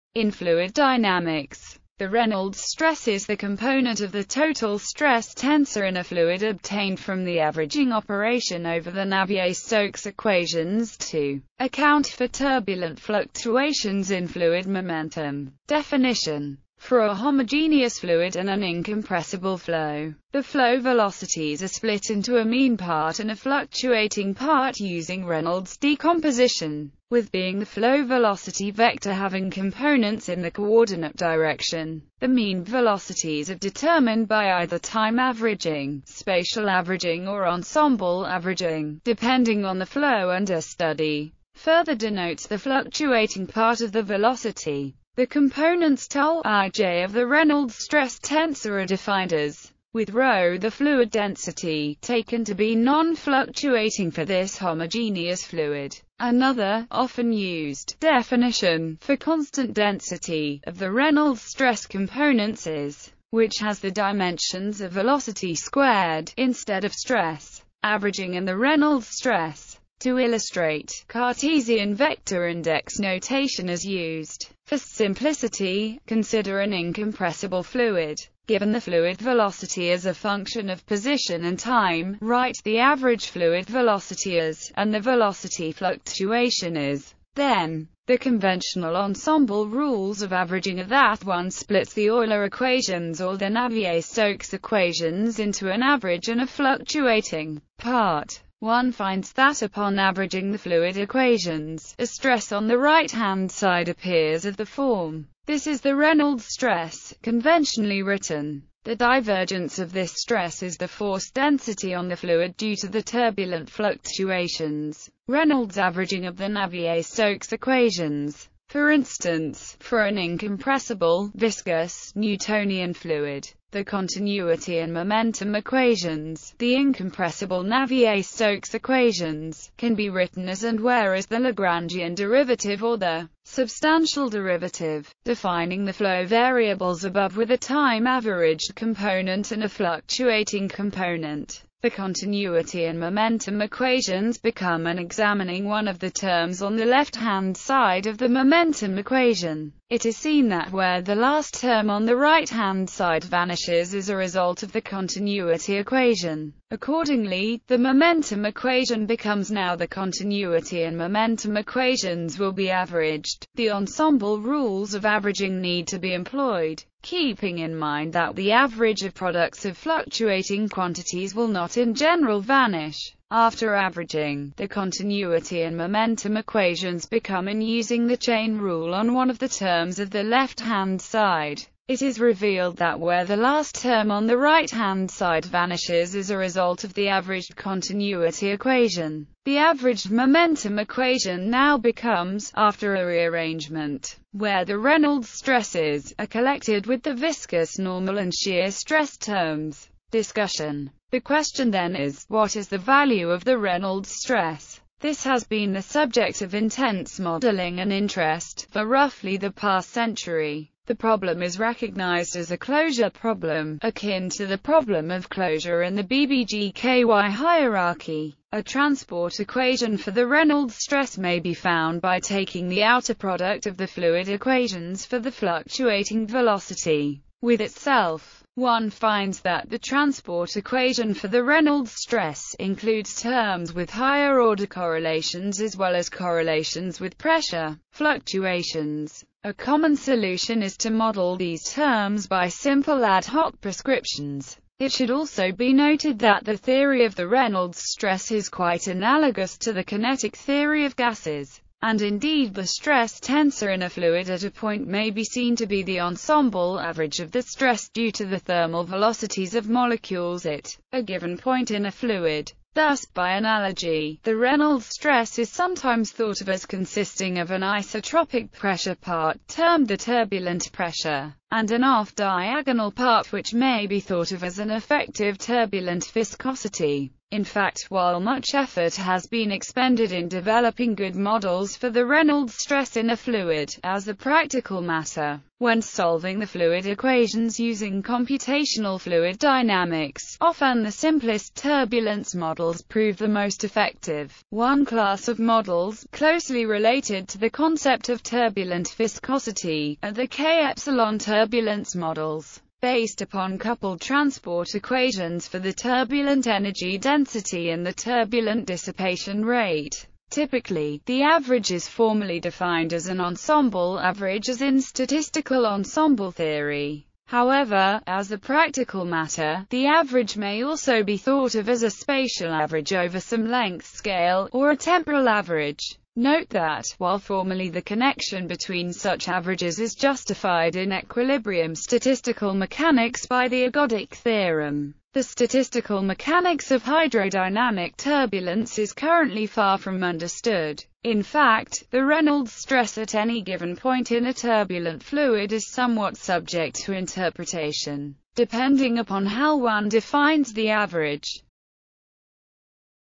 Reynolds stress
In fluid dynamics, the Reynolds stress is the component of the total stress tensor in a fluid obtained from the averaging operation over the Navier-Stokes equations to account for turbulent fluctuations in fluid momentum.